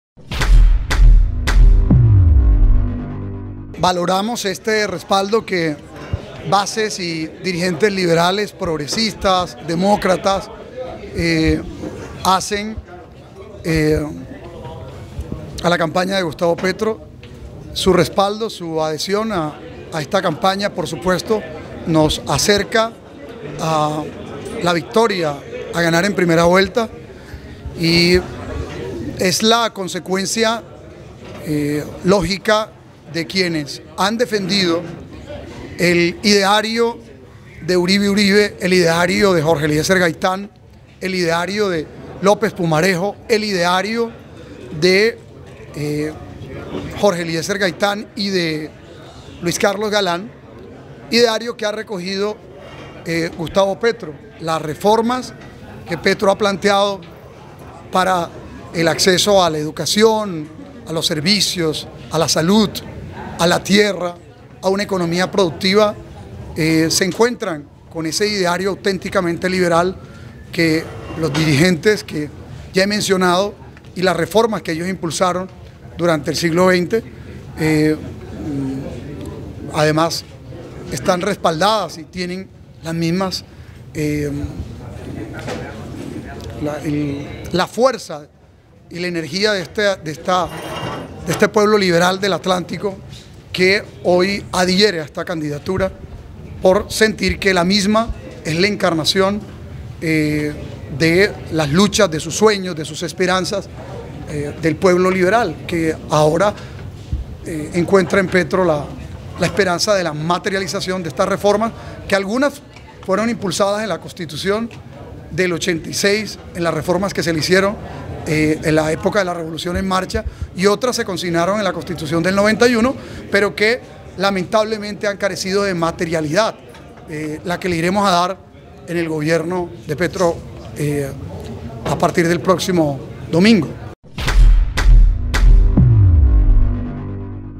Bases liberales del Atlántico se suman a la candidatura de Gustavo Petro
Quedó protocolizada la llegada de las bases liberales del Atlántico a la candidatura de Gustavo Petro, en un evento que fue presidido por el exalcalde samario Carlos Caicedo Omar.

Los militantes liberales que asistieron al evento, muchos de ellos representantes de sectores afines a la izquierda democrática, resaltaron que Petro ha recogido las ideas auténticas del “trapo rojo”, como se le conoce al liberalismo más tradicional.